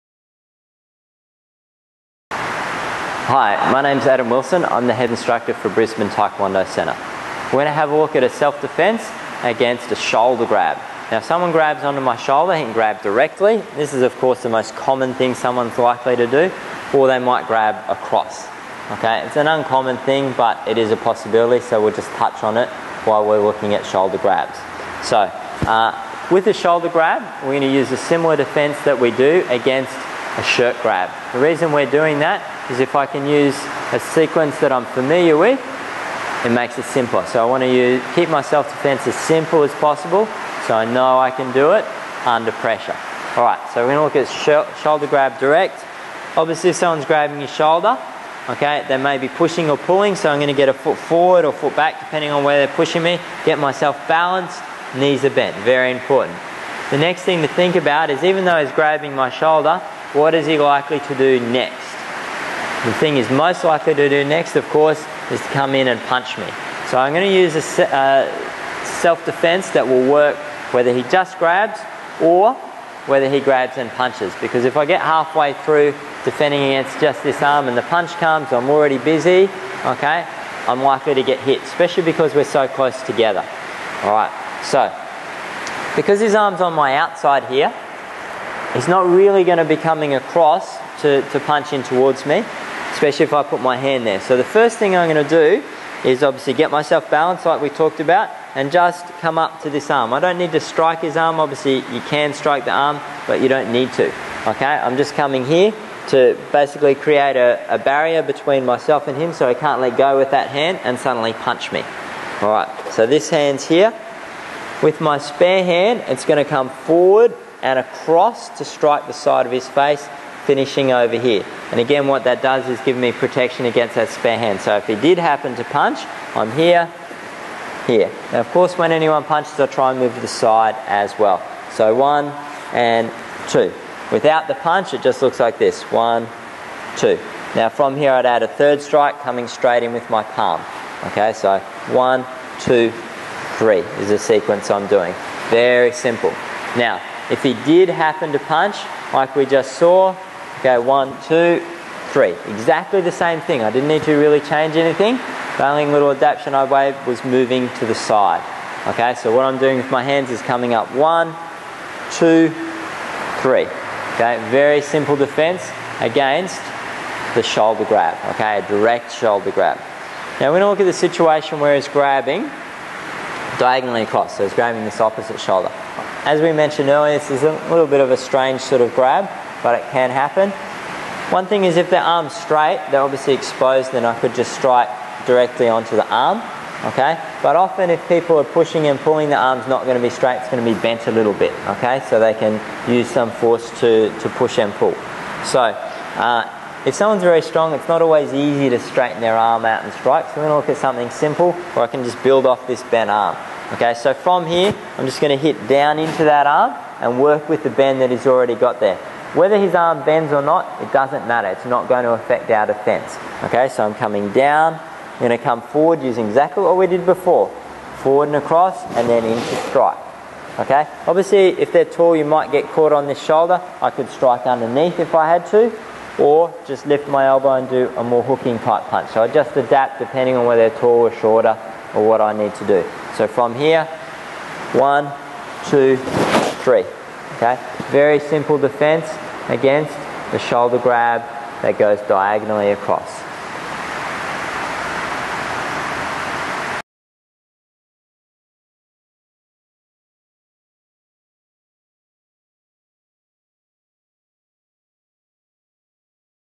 Self Defence - Shoulder Grab from Front from Brisbane Taekwondo Centre
This video shows a simple and practical self defence against a shoulder grab from the front ::: BTC Ph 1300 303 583 ::: Come and try a class today!